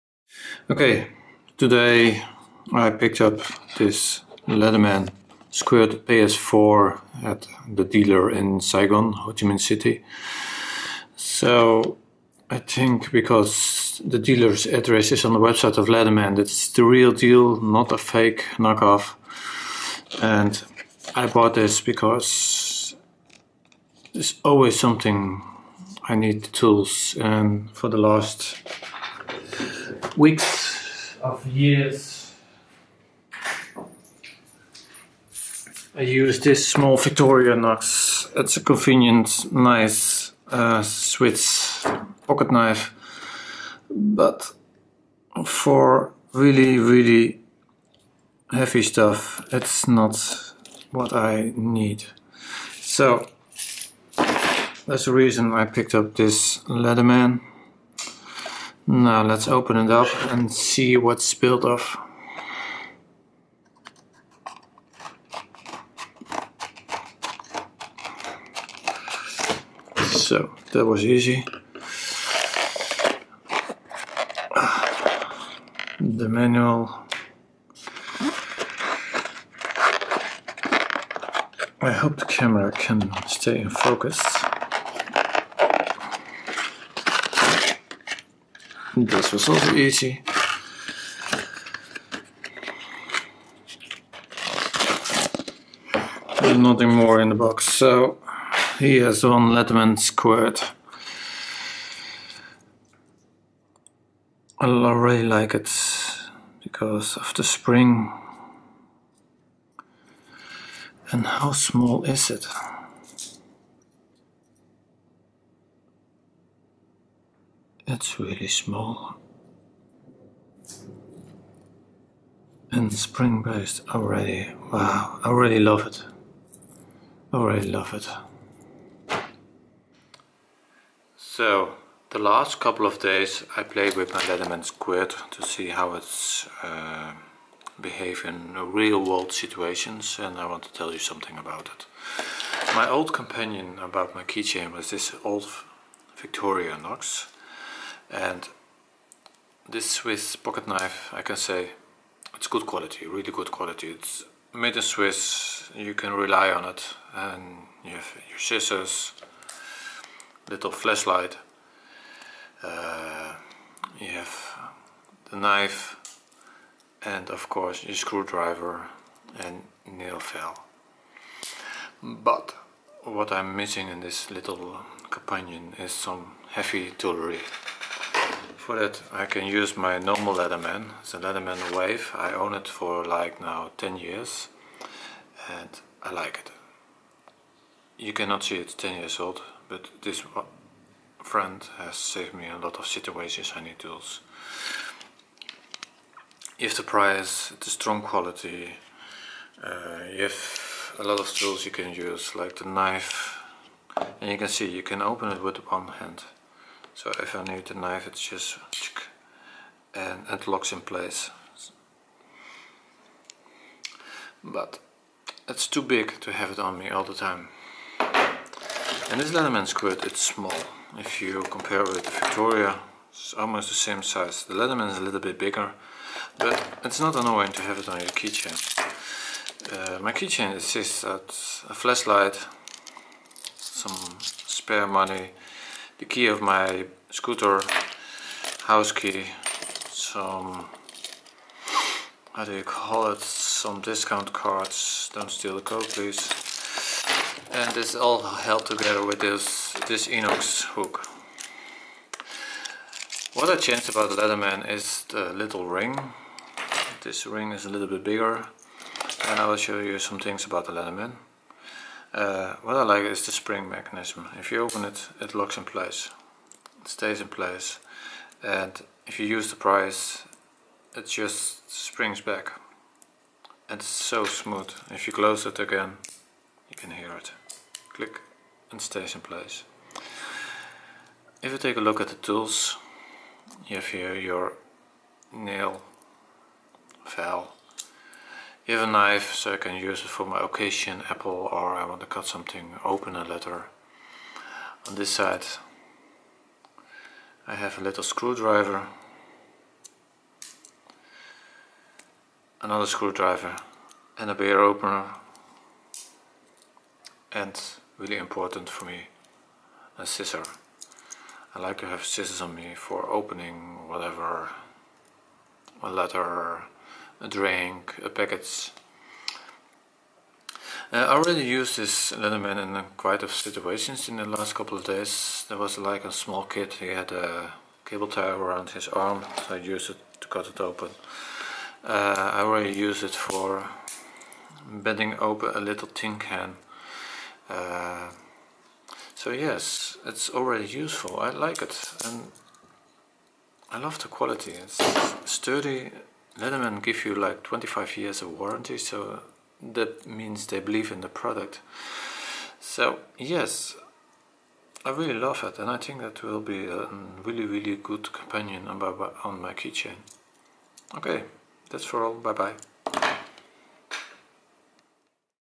Leatherman Squirt PS4 mini unpack and review. The best EDC tool for everyday​ use.
The streets of Saigons. You need tools on those streets. This city break everything. So its nice to have some tools on me. The Leatherman Squirt PS4 is small enough to be carried on a keychain.